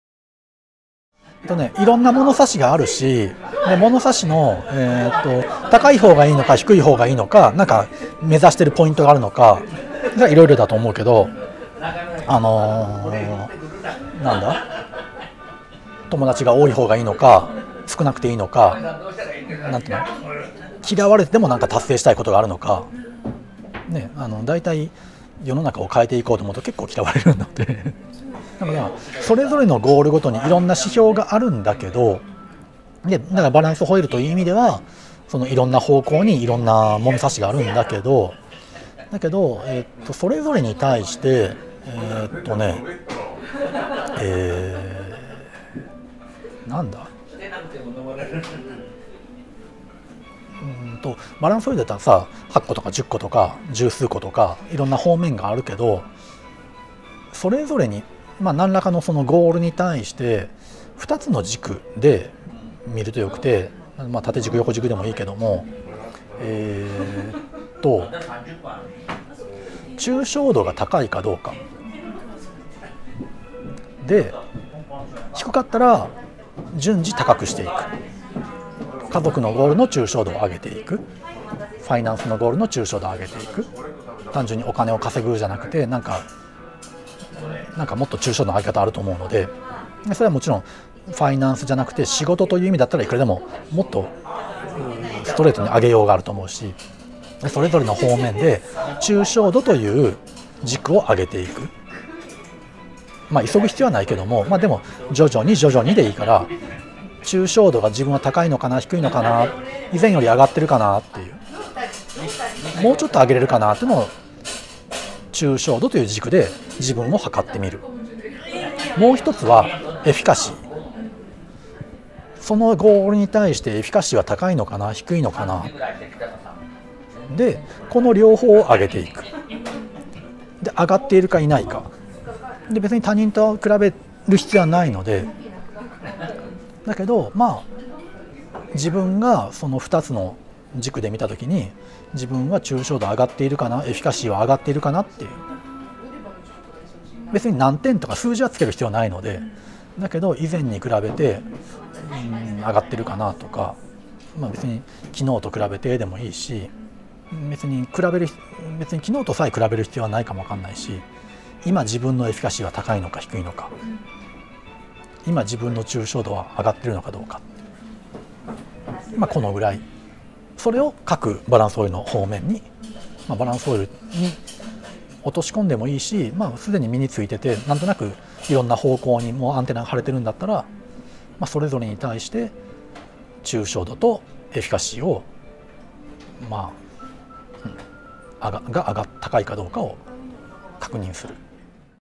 [質問] 自分の成長を測る基準はありますか？
とある講座の開催後、
受講生から質問を頂きました。

「自分の成長を測る基準はありますか？」

自分を測る物差しは、他人が決めるものではありません。
ただし、参考としてお応えするならば、「抽象度とエフィカシー」です。

もちろん、ゴールは人それぞれ。
ご自身のゴールについて、「抽象度とエフィカシー」が高いのか。
時折、確認していただくといいでしょう。

尚、この時、開催した講座は、こちらです↓

「コーチング講座(初級) スコトーマのコントロール術」
スコトーマ（心理的盲点）が生まれる仕組みと、そのコントロール術。

苫米地式コーチング認定コーチ　坂本裕史

(フォローはご自由に。友達申請は、自己紹介を添えて。）